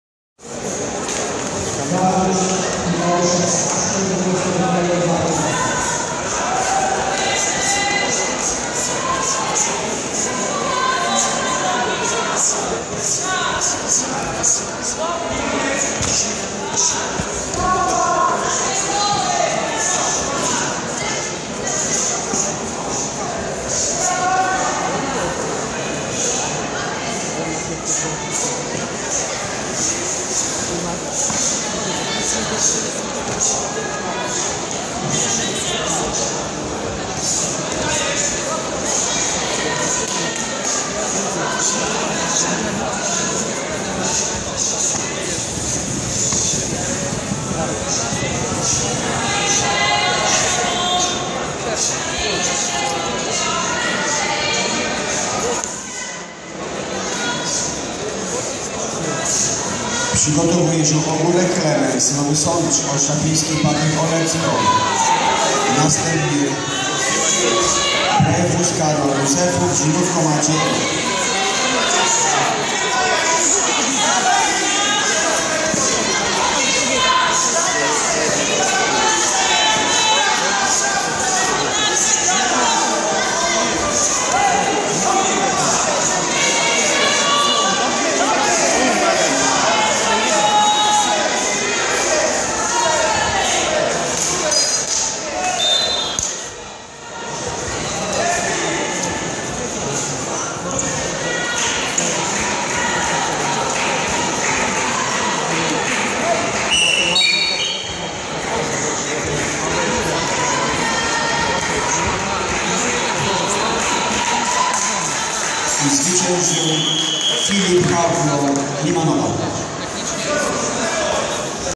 Puchar Polski Karate Kyokushin 2015 - młodzicy, półfinał
Puchar Polski Seniorów, Młodzików i Juniorów Młodszych - Gdańsk 2015.
Kumite młodzików -65 kg, półfinał.

12 grudnia 2015r.